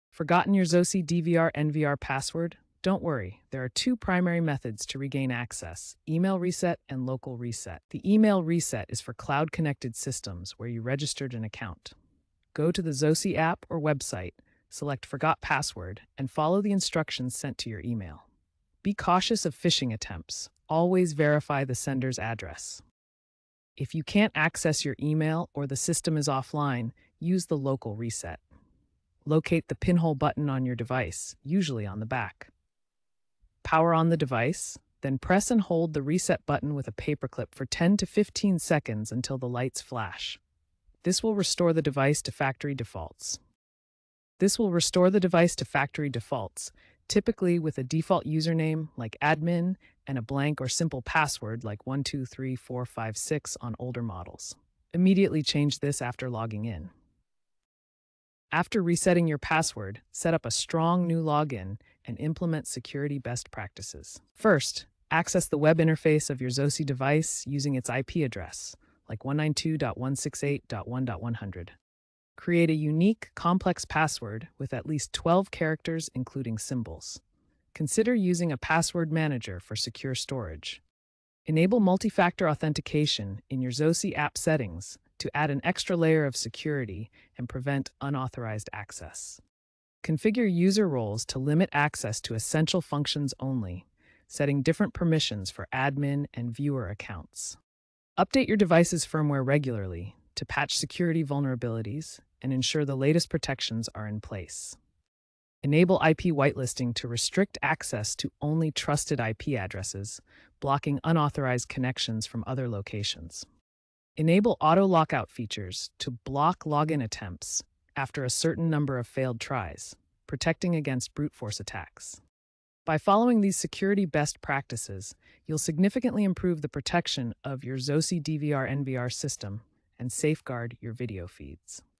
How To Reset Your Zosi DVR/NVR Password – Email/Local Reset & New Login Setup
Reset your Zosi DVR/NVR password effortlessly with our detailed instructions. Explore both email and local reset options for a seamless login experience.